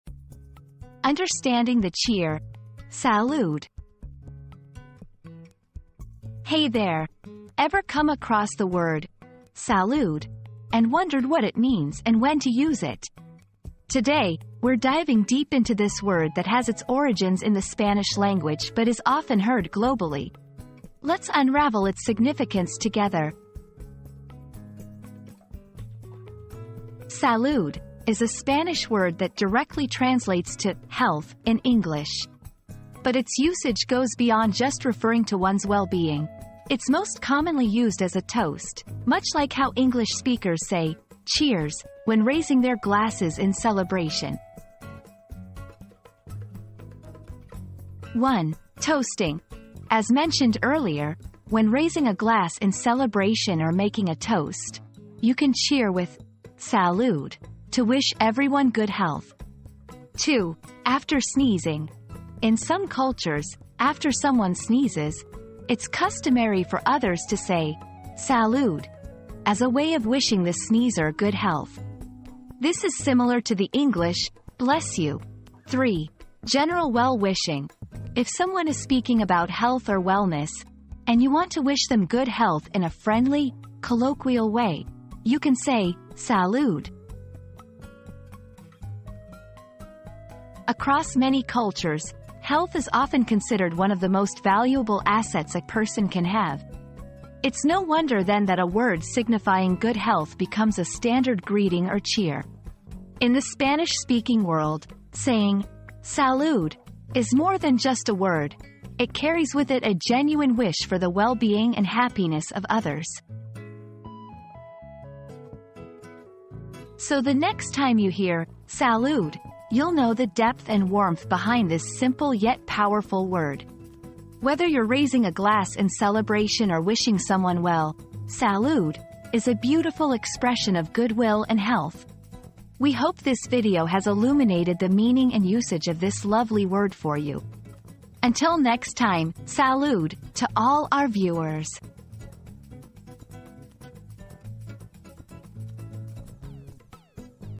Understanding the Cheer "Salud!"
Unraveling the Significance of the Cheer 'Salud!' • Discover the meaning and cultural significance behind the cheer 'Salud!' and learn when and how to use it. Dive deep into this powerful word that signifies good health and well-being, and understand its importance in different cultures. Join us as we explore the depth and warmth behind this simple yet impactful word.

00:00 • Introduction - Understanding the Cheer "Salud!"
00:26 • What Does "Salud!" Mean?
00:47 • When to Use "Salud!"
01:30 • The Cultural Significance of "Salud!"